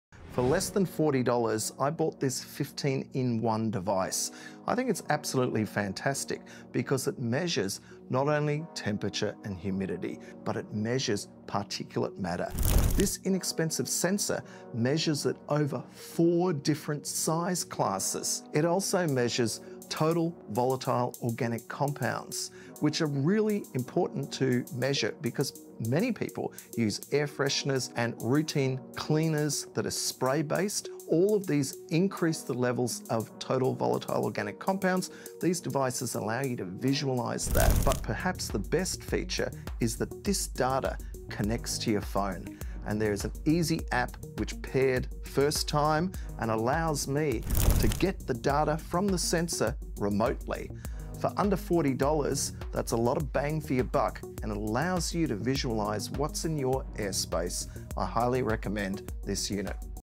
For Less than $40 I Can Track My Indoor Air Quality From My Phone
This $40 device shows you exactly what’s in your air 🌫️📱

I picked up a 15-in-1 indoor air quality monitor - and it’s honestly a game changer. It tracks:
✅ Temperature & humidity
✅ 4 sizes of particulate matter
✅ Total volatile organic compounds (TVOCs)
TVOCs can spike from everyday products like air fresheners and cleaning sprays. This device shows it all - in real time - right on your phone.
For under $40, it’s a powerful tool to understand and protect your home environment.